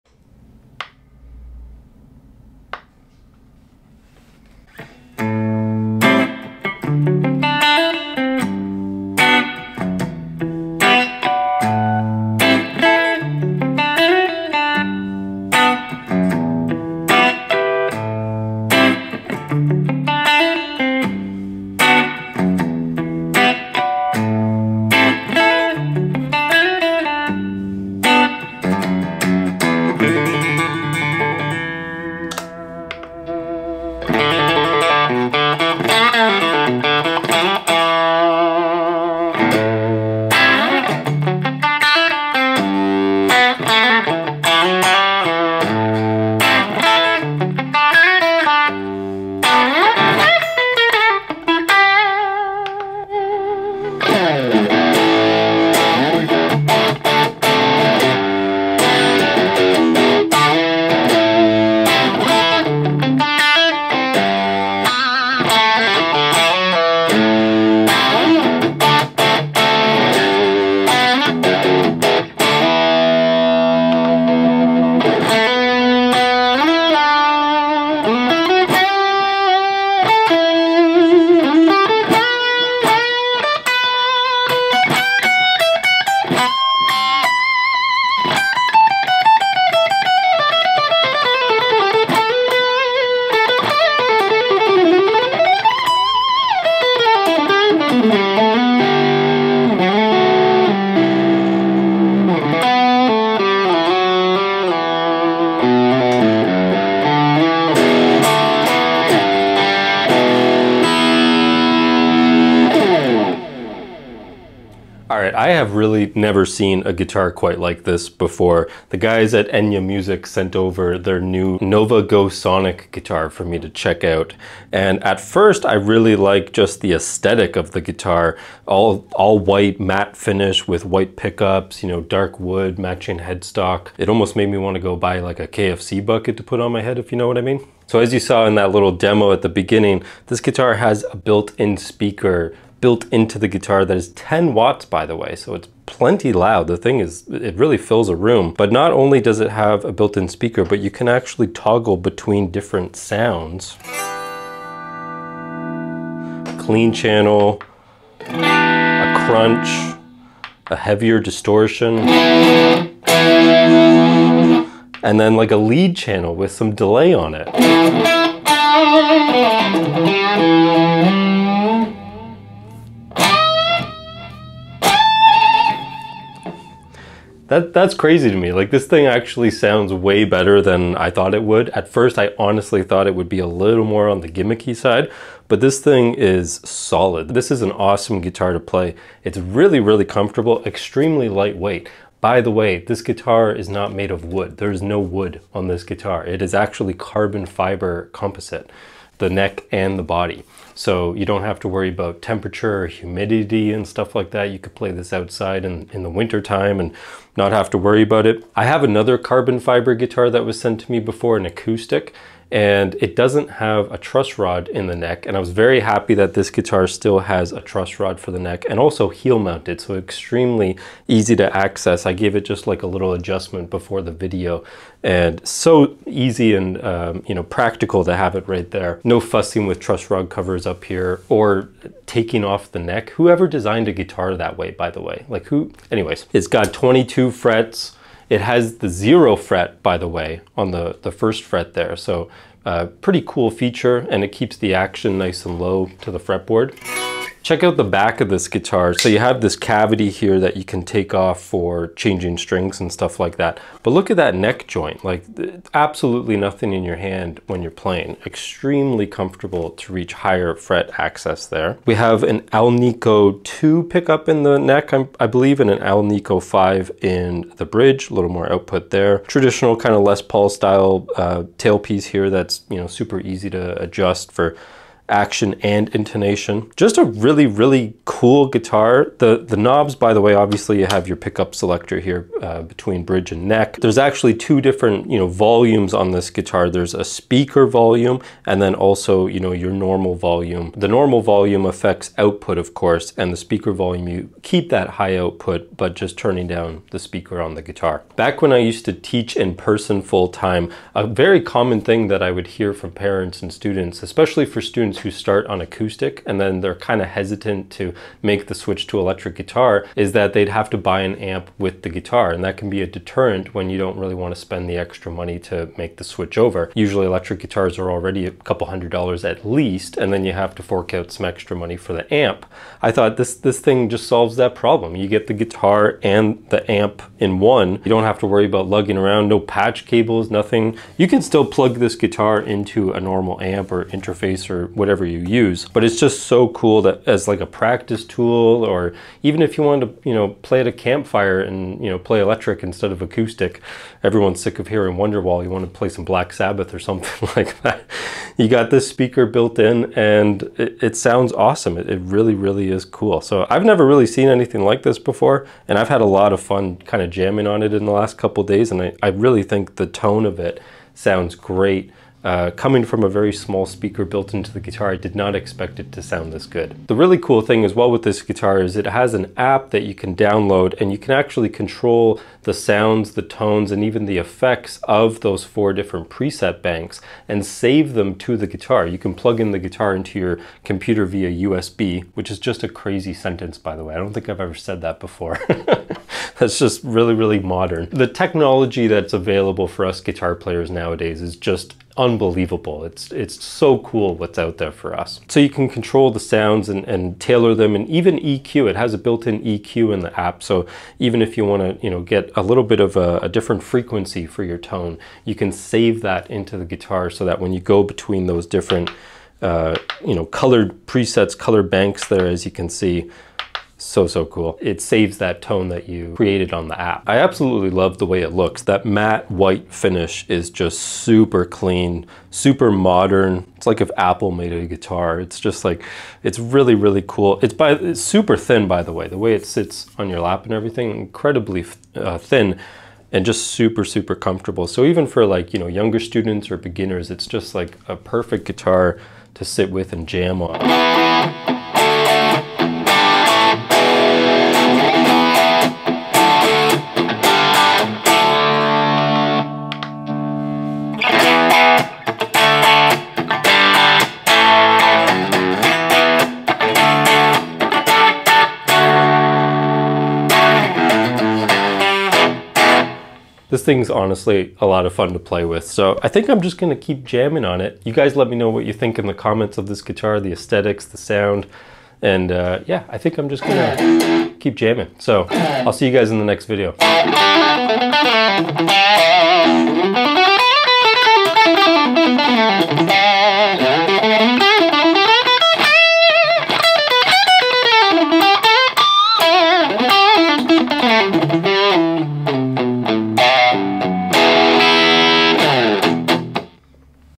NO AMP?? NO PROBLEM!
Today we are checking out the Nova Go Sonic from @enyamusicglobal!

This is such a cool guitar and a GREAT option for students, people who travel, and players who want a high quality compact set up!

You can control and customize your tones with the Enya Music app! Adding effects, EQ, and more to each preset. You can even play backing tracks through the guitar and control the volume of your guitar and the backing track separately!

This guitar definitely impressed me and is a lot of fun to play!

0:00 Intro
1:54 Great or Gimmick?
3:12 No Wood?
4:07 Back Cavity + Neck Joint
4:21 Pickups + Bridge
4:39 Volume Controls
4:58 A Common Problem For Students (Solved!)
6:07 Control The Tone With Your PHONE!
6:57 Final Thoughts